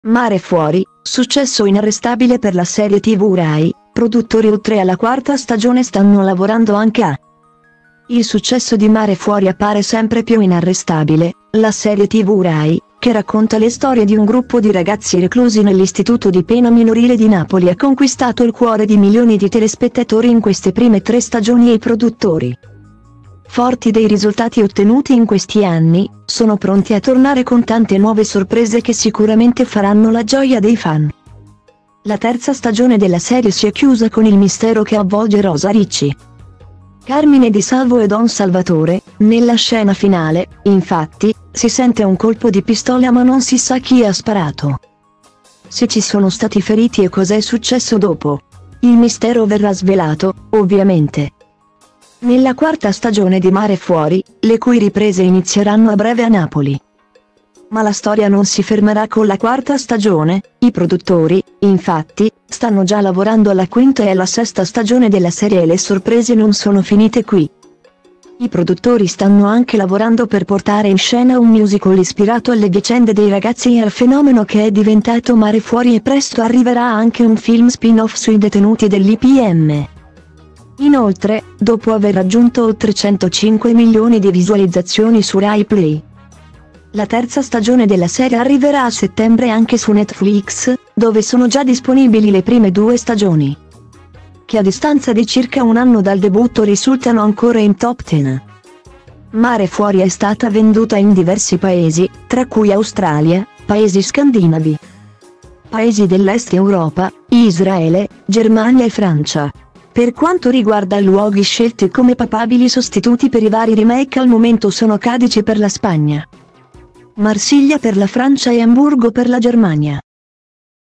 Mare Fuori, successo inarrestabile per la serie tv Rai: produttori oltre alla 4ª stagione stanno la
Mare Fuori, successo inarrestabile per la serie tv Rai:  produttori oltre alla 4ª stagione stanno lavorando anche a…”
Il successo di Mare Fuori appare sempre più inarrestabile: la serie tv Rai, che racconta le storie di un gruppo di ragazzi reclusi nell’Istituto di Pena Minorile di Napoli ha conquistato il cuore di milioni di telespettatori in queste prime tre stagioni e i produttori,forti dei risultati ottenuti in questi anni, sono pronti a tornare con tante nuove sorprese che sicuramente faranno la gioia dei fan. La terza stagione della serie si è chiusa con il mistero che avvolge Rosa Ricci,